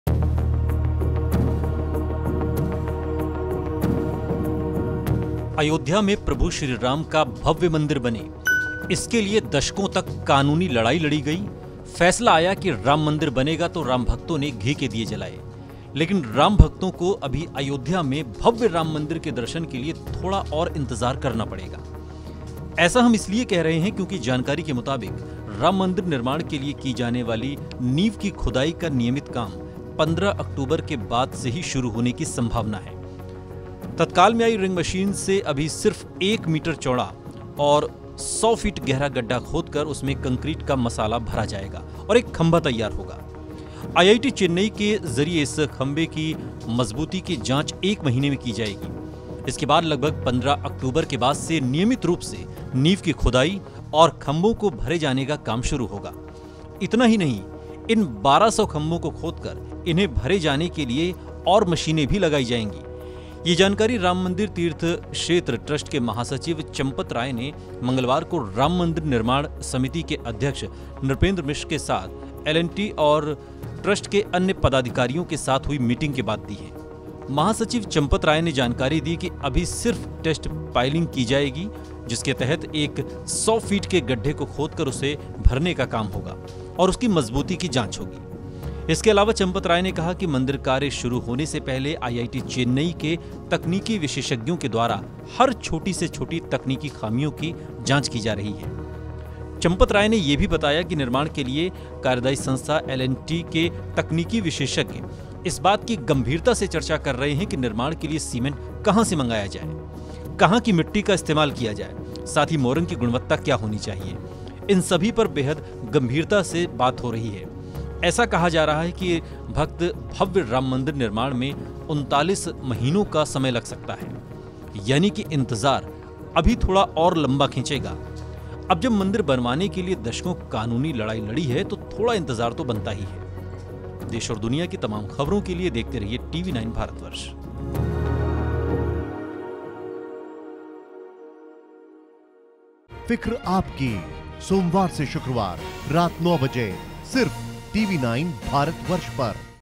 Ayodhya Ram Mandir : जानिए क्यों रूका भक्तभव्य मंदिर निर्माण कार्य ?
अयोध्या में प्रभु श्रीराम का भव्य मंदिर बने इसके लिए दशकों तक कानूनी लड़ाई लड़ी गई. लेकिन राम भक्तों को अभी अयोध्या में भव्य राम मंदिर के दर्शन के लिए थोड़ा और इंतजार करना पड़ेगा. क्योंकि नीव की खुदाई का काम 15 अक्टूबर के बाद शुरू होने की संभावना है.